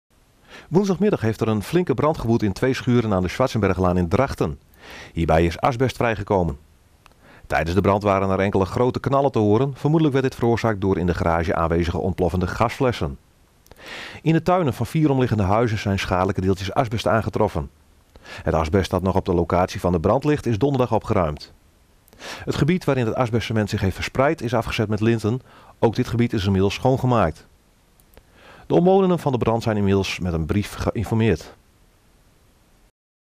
GPTV: Felle brand verwoest garage in Drachten
Woensdagmiddag heeft er een flinke brand gewoed in twee schuren aan de Schwartzenberghlaan in Drachten. Hierbij is asbest vrijgekomen. Tijdens de brand waren er enkele grote knallen te horen. Vermoedelijk werd dit veroorzaakt door in de garage aanwezige ontploffende gasflessen. In de tuinen van vier huizen zijn de schadelijke deeltjes aangetroffen. Het asbest dat nog op de locatie van de brand ligt, is donderdag opgeruimd. Het gebied waarin het asbestcement zich heeft verspreid, is afgezet met linten. Ook dit gebied is inmiddels schoongemaakt. De omwonenden van de brand zijn inmiddels met een brief geïnformeerd.